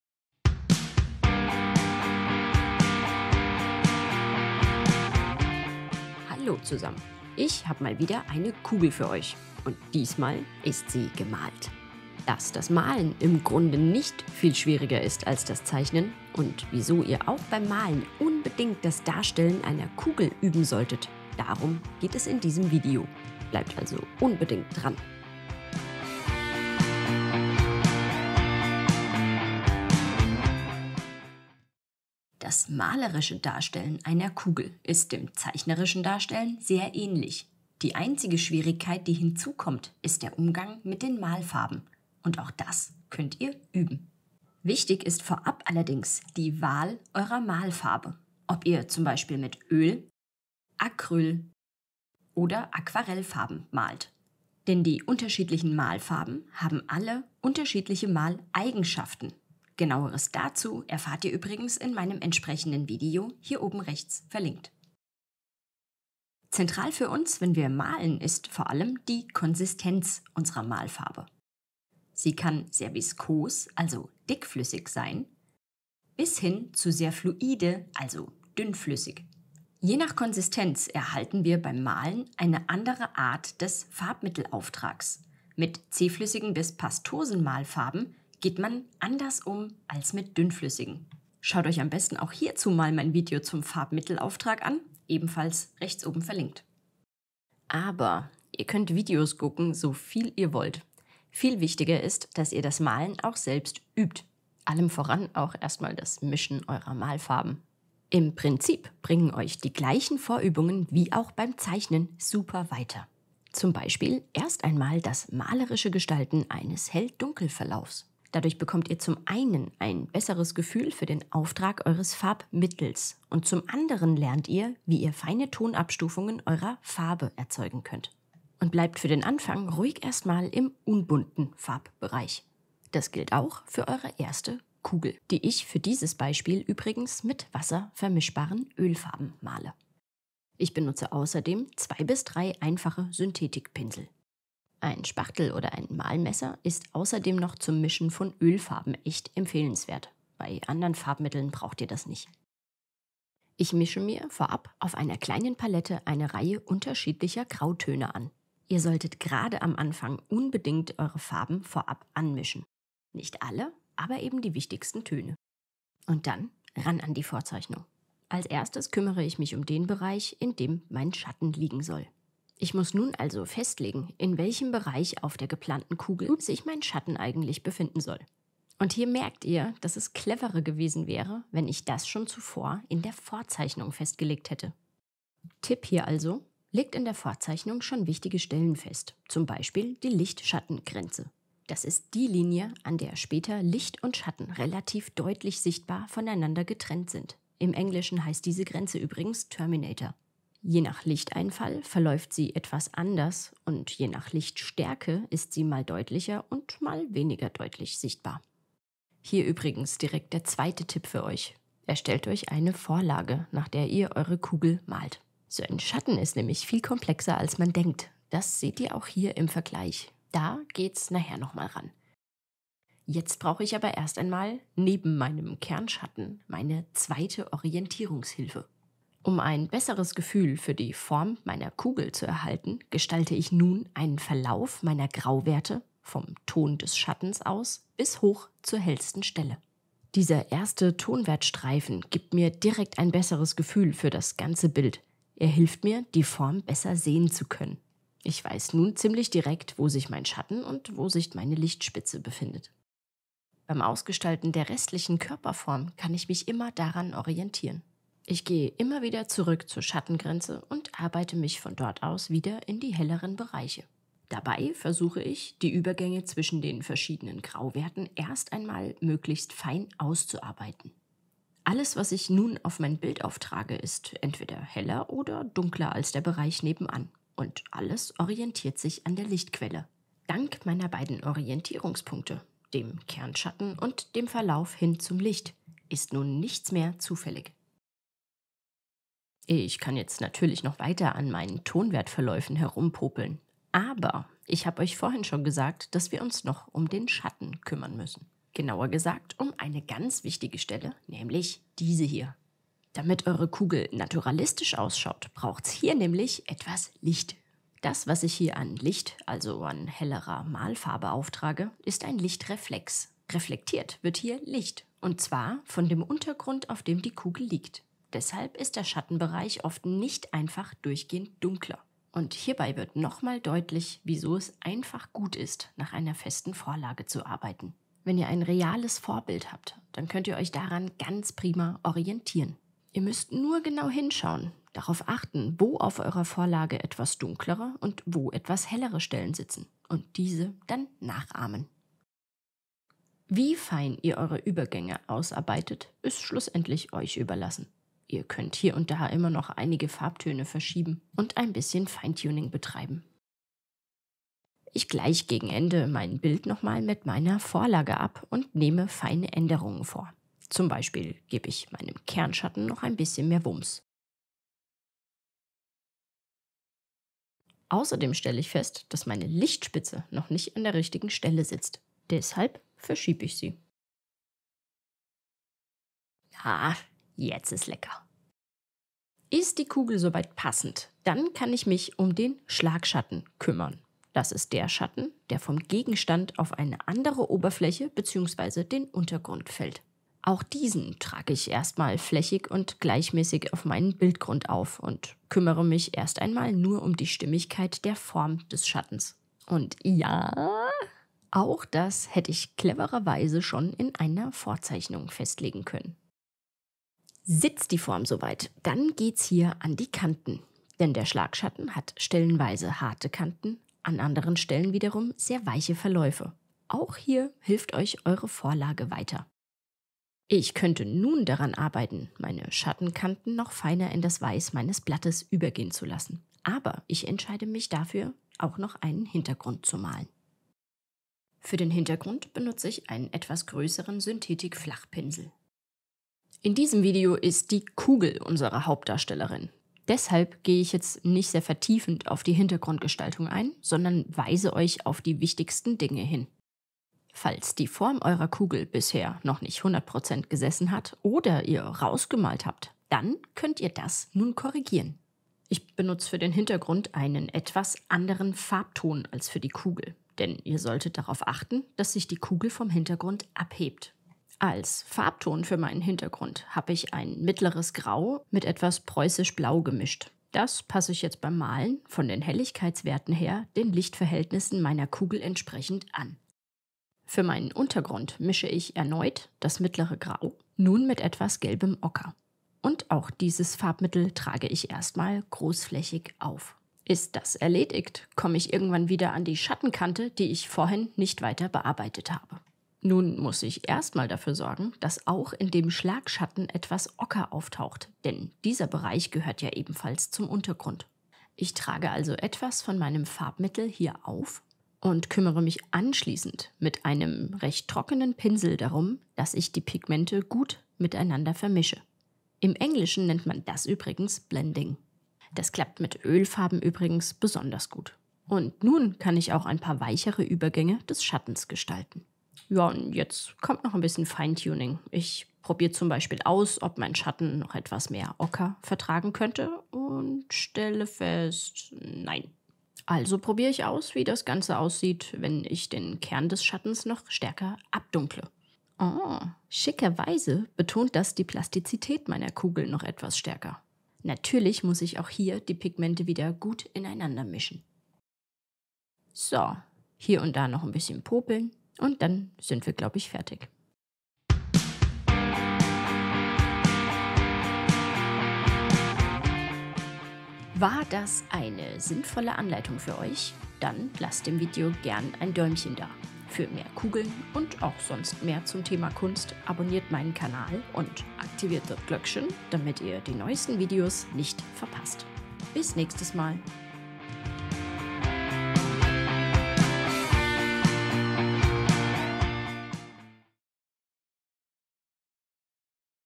Grundlagen Malerei – Eine Kugel malen
Ihr möchtet eine Kugel malen – diese also möglichst genau in ihrer Schattierung gestalten, sodass sie plastisch und damit wie ein Abbild aus der räumlichen Welt erscheint. Wie ihr das am besten macht und worauf genau ihr dabei achten müsst, erfahrt ihr in diesem Video.

Um ein besseres Verständnis für die einzelnen Bereiche von Licht und Schatten und ein besseres Gefühl für deren Darstellung zu erhalten, solltet ihr unbedingt zuvor das Video „Grundlagen Zeichnen – Eine Kugel zeichnen“ angesehen haben. Ihr findet es unter folgenden Link:

Ich kann immer wieder das Zeichnen und Malen von Kugeln empfehlen. Übt dabei erst einmal, eine Zeichnung anzulegen und auszuarbeiten. Dabei lernt ihr den Umgang mit Licht und Schatten, also mit Hell- und Dunkel-Werten (Tonwerten). Geht am besten dann erst in die malerische Gestaltung.

INHALTE

Vorbemerkungen:
0:45 Auswahl der Malfarbe
1:07 Konsistenz der Malfarbe
1:30 Farbmittel-Auftrag
1:40 Vorübungen: Mischen & Tonwert-Verlauf

Eine Kugel malen:
2:15 Material
2:35 Palette anlegen: Farbtöne mischen
2:49 Kernschatten anlegen
3:00 Licht-Schatten-Grenze festlegen
3:32 Nach einem Vorbild arbeiten
3:48 Tonwert-Streifen anlegen
4:15 Restliche Körperform ausarbeiten
5:00 Der Reflex
5:43 Feinheiten ausarbeiten
6:22 Schlagschatten anlegen
6:50 Feinheiten der Kanten bearbeiten
7:10 Hintergrund anlegen
7:58 Grundfläche anlegen